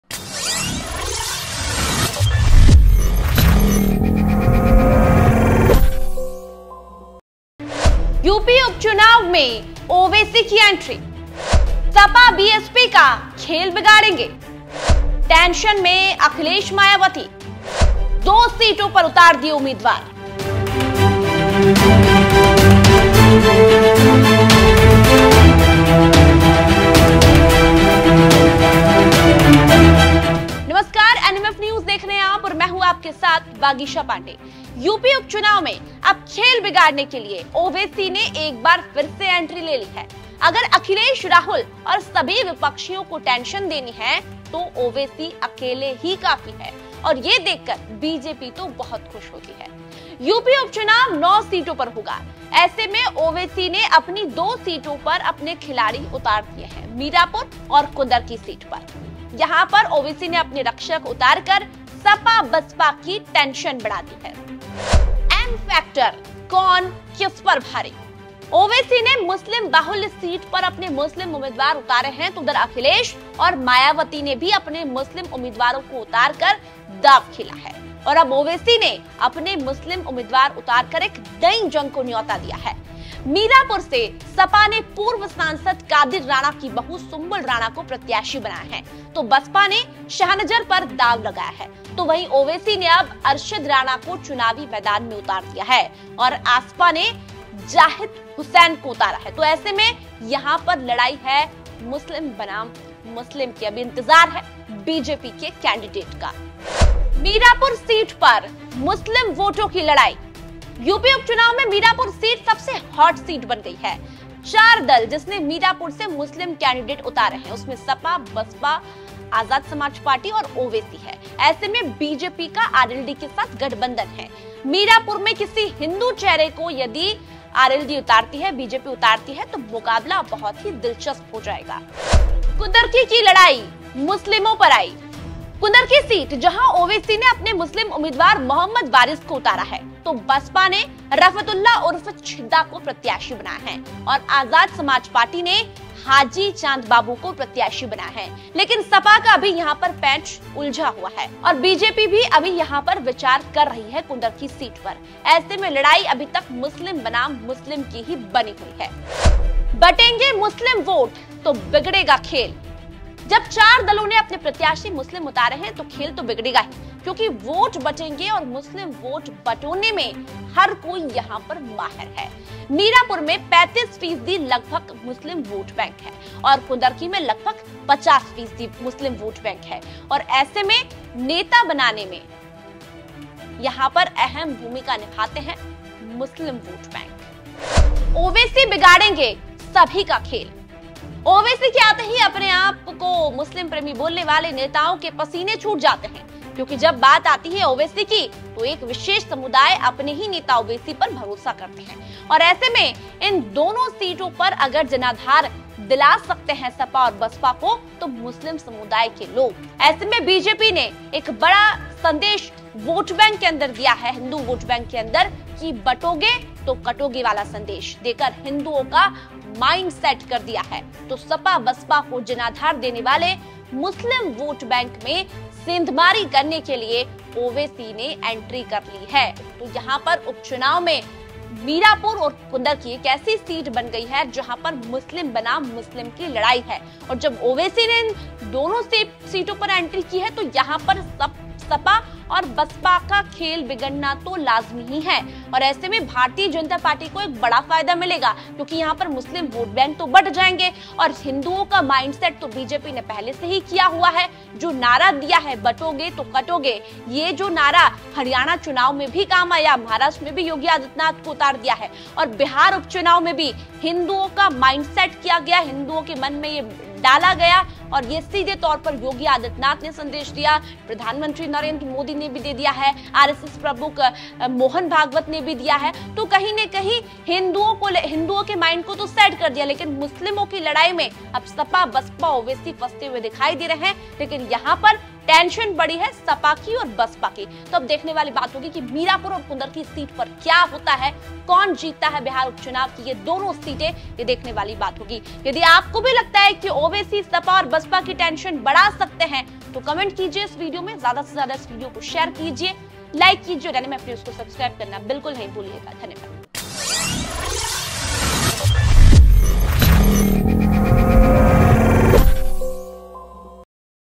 मुस्लिमों पर Owaisi ने किया इतना भयंकर ‘खेला’, अब क्या करेंगे Akhilesh ?
ओवैसी ने मुस्लिम बाहुल्य सीट पर अपने मुस्लिम उम्मीदवार उतारे है तो उधर अखिलेश और मायावती ने भी अपने मुस्लिम उम्मीदवारों को उतारकर दांव खेला है , और अब ओवैसी ने अपने मुस्लिम  उम्मीदवार उतार कर एक नई  जंग को न्योता दिया है  .

vagisha
fazl